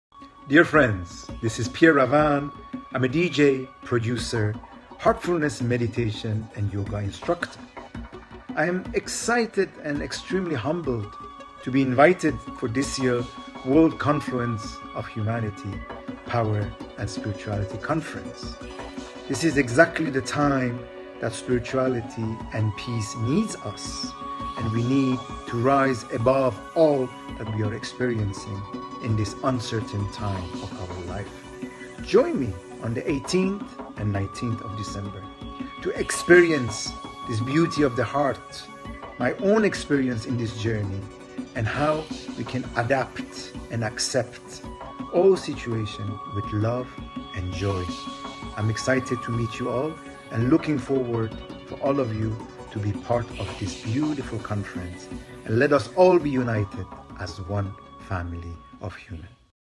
13th World Confluence of Humanity, Power & Spirituality
Join us in the 13th World Confluence of Humanity, Power & Spirituality. A platform where Spirituality ignites Humanity.
Eminent Spiritual & Motivational leaders from across the world.